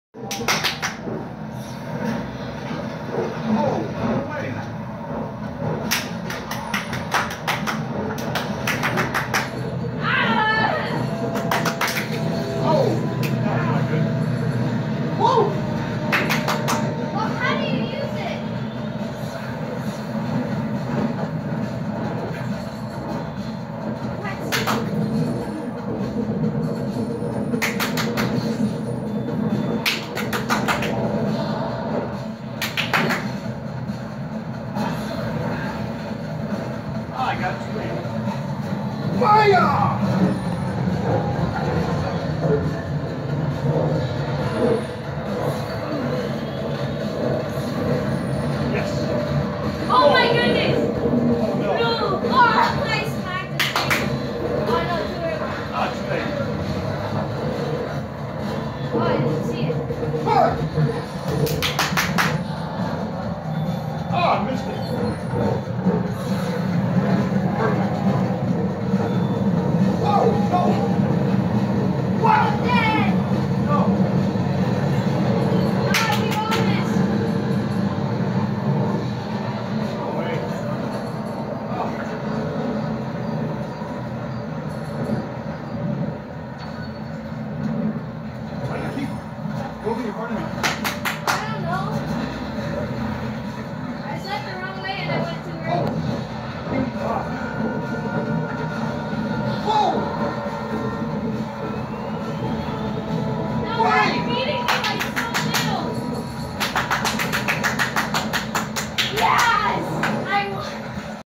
father & son on virtual bike racing (the AmeriPinos)
Saturday night bonding playing bike racing